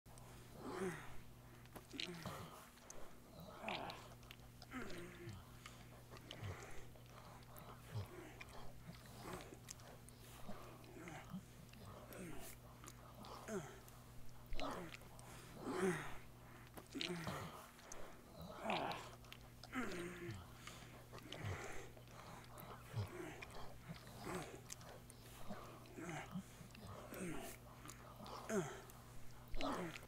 Big Bad Wolf
Is Jax just that sweet or what? The big bad wolf just can’t get enough of him!

15 second animation loop complete with sound by Audio Elk.